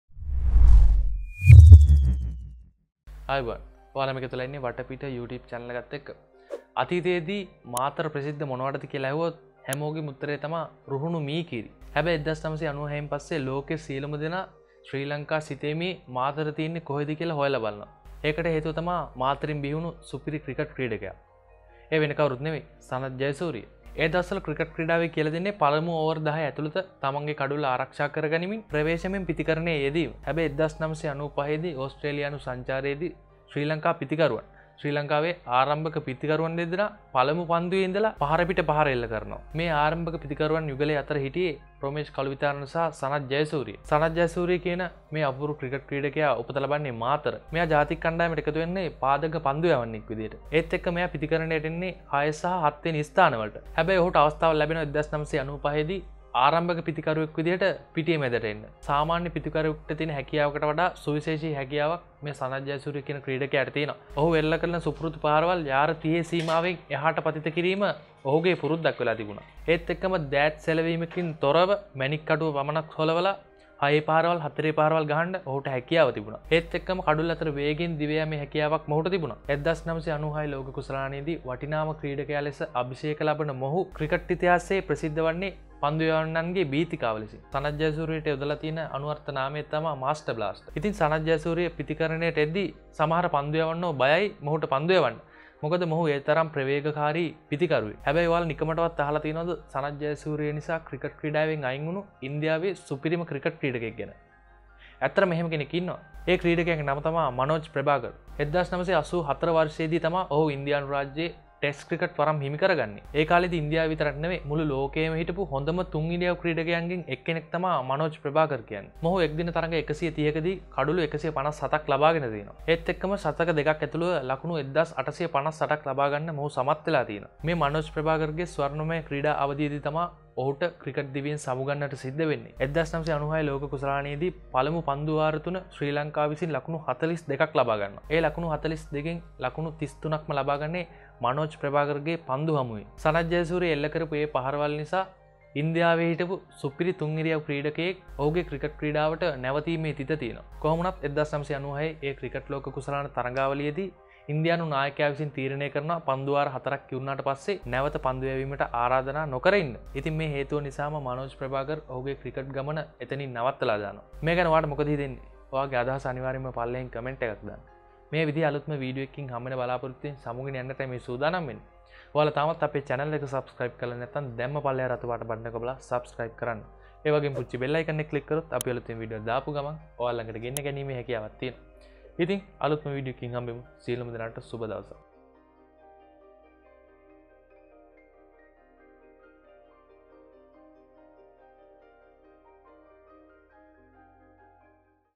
අංක එකේ තුන් ඉරියවු ක්‍රීඩකයාව ගෙදර යවපු හැටි
එක්දින තරඟ 130කදී කඩුලු 157 සහ ශතක දෙකක් සහිතව ලකුණු 1858ක් ලබාගෙන සිටි මොහුට තමන්ගේ ක්‍රිකට් දිවියේ ස්වර්ණමය කාලයේම සමුගන්නට සිදුවුයේ සනත් ජයසුරියේ ප්‍රවේගකාරී පිතිකරණය නිසාය